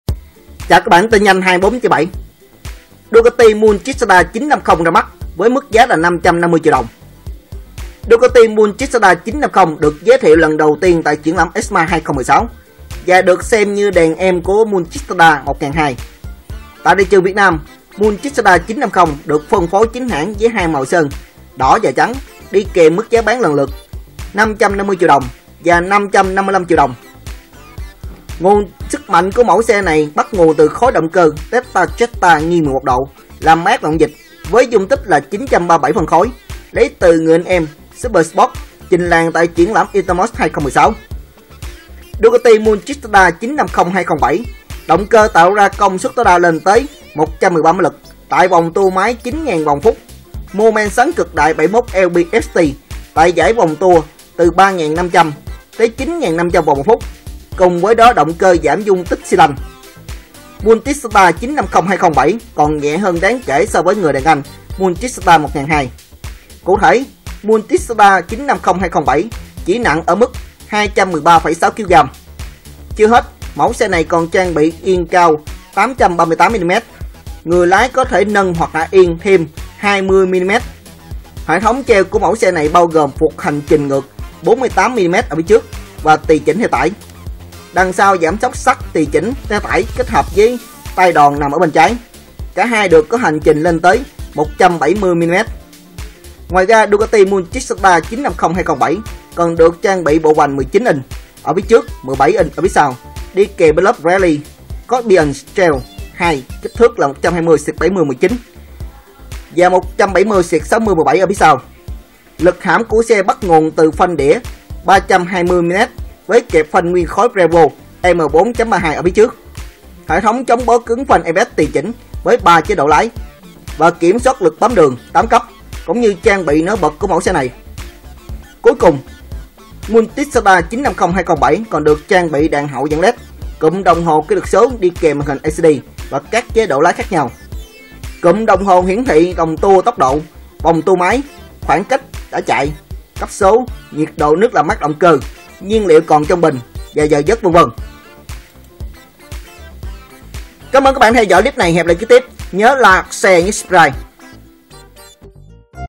Tin nhanh 24/7 - Ducati Multistrada 950 ra mắt, giá từ 550 triệu Đồng
Ducati Multistrada 950 2017 sử dụng khối động cơ Testastretta nghiêng 11 độ, dung tích 937 phân khối lấy từ người anh em SuperSport sản sinh công suất tối đa 113 mã lực.